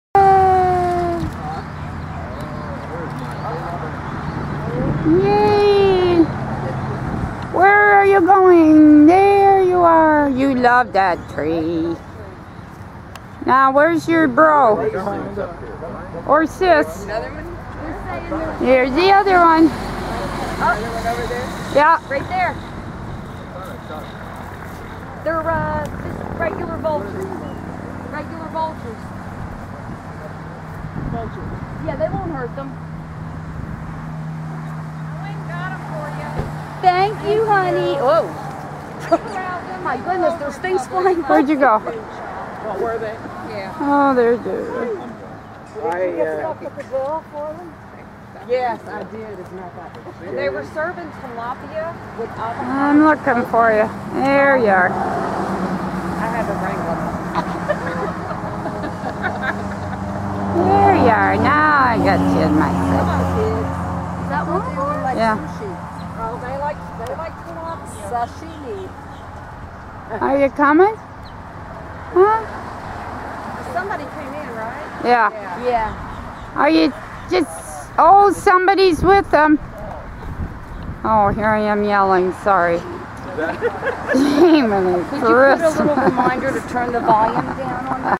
SW Florida Eagles_One Of The E's Return Flight From The River 4-20-13
One and a half hours after both Hope and Honor flew to the river, one returns. Their flying skills are impressive. Thank you Dick Pritchett Real Estate for maintaining such a suitable tree and environment for our Bald Eagle Family.